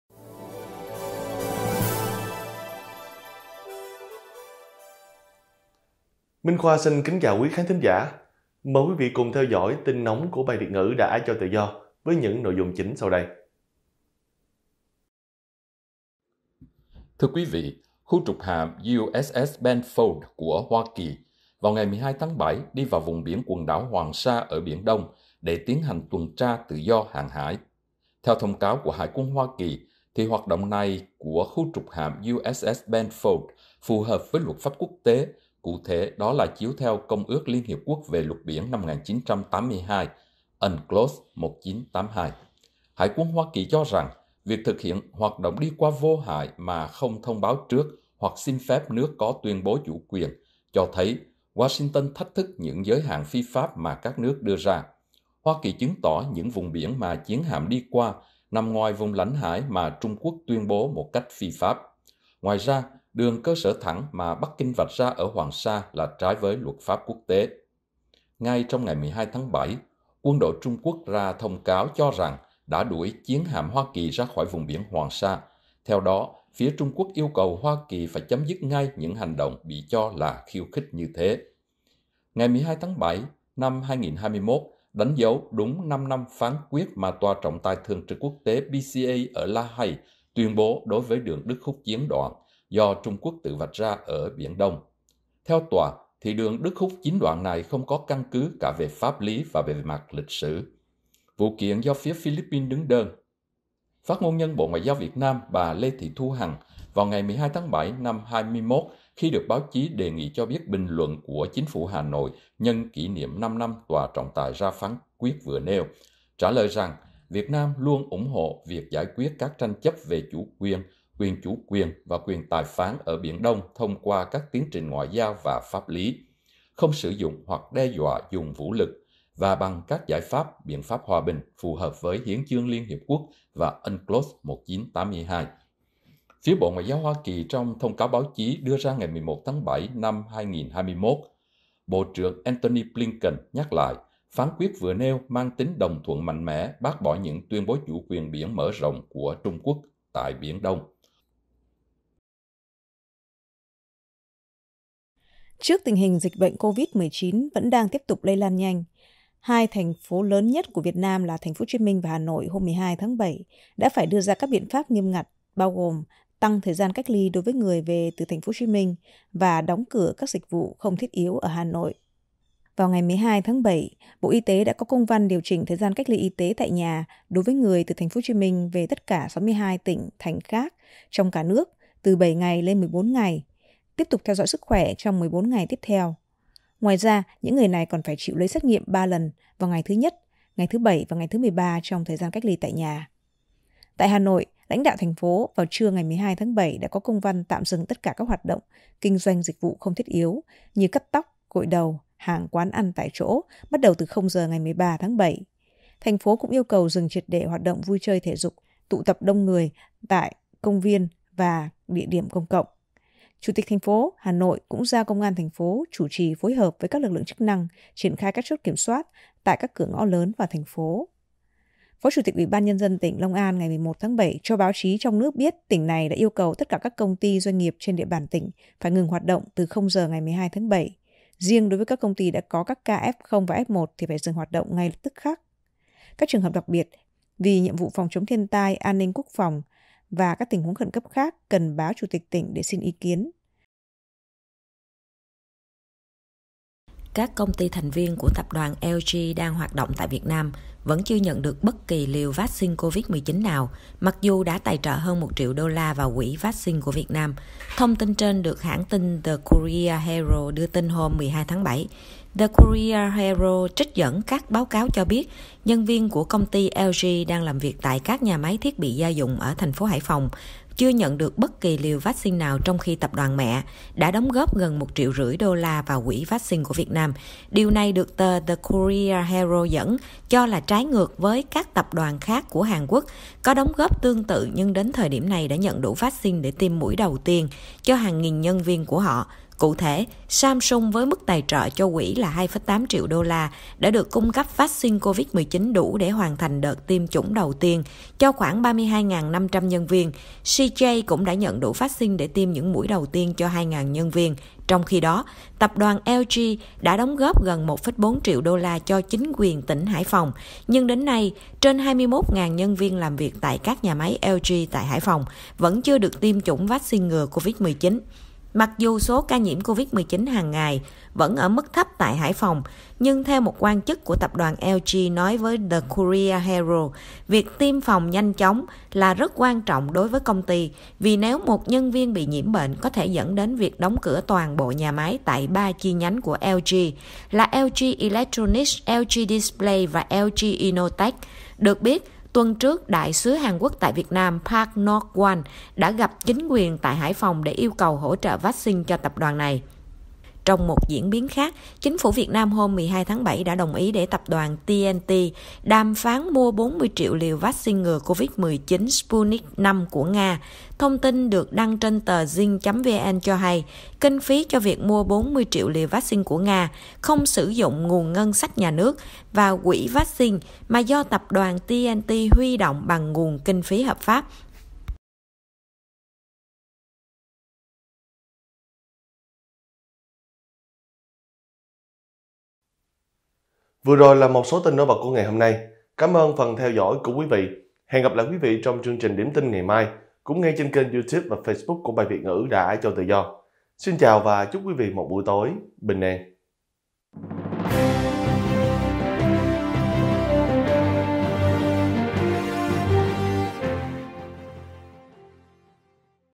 Chiến hạm Hoa Kỳ đi vào vùng biển Hoàng Sa, Trung Quốc nói đã xua đuổi đi
1. Chiến hạm Hoa Kỳ đi vào vùng biển Hoàng Sa, Trung Quốc nói đã xua đuổi đi
2. Hà Nội ngừng hoạt động các dịch vụ không thiết yếu
3. Tập đoàn LG chưa được hỗ trợ vắc-xin ngừa COVID-19 mặc dù đã tài trợ hơn 1 triệu đô cho Quỹ vắc-xin